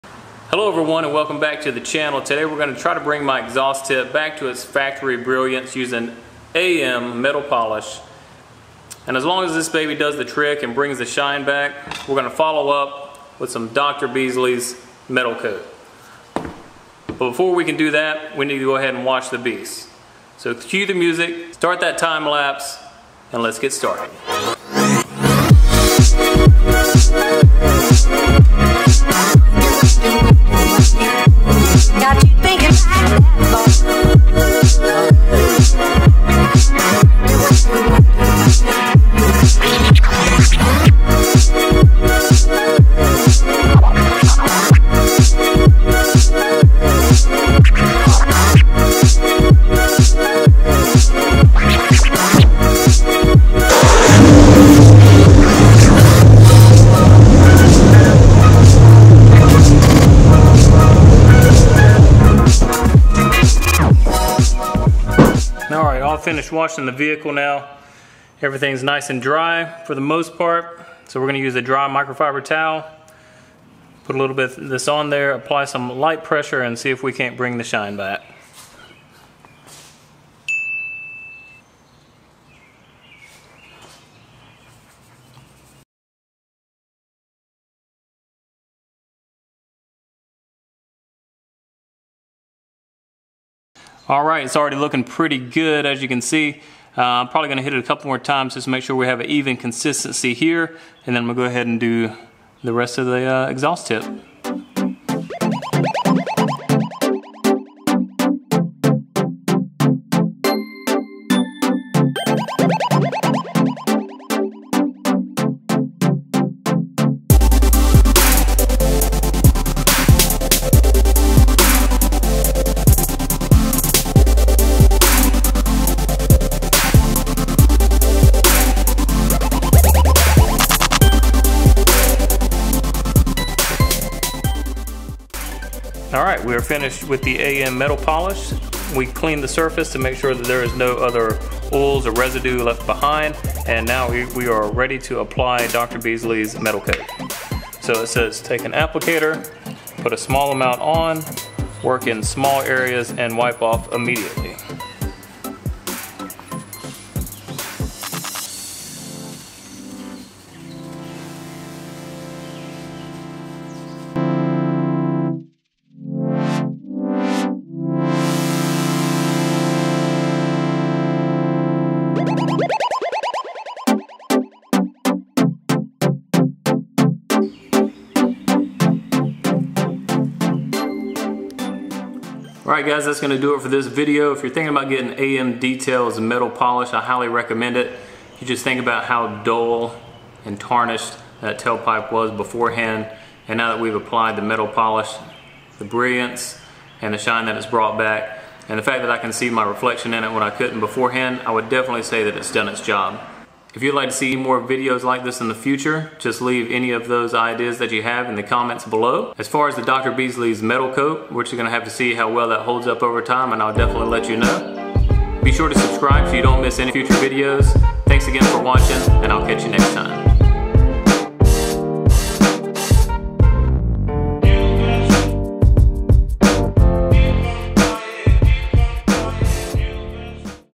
Trying out AMDetails Metal Polish and Dr. Beasley's Metal Coat!!
Testing AMDetails Metal Polish on my Magnaflow exhaust tip and protecting it with Dr. Beasley's Metal Coat. The results speak for themselves.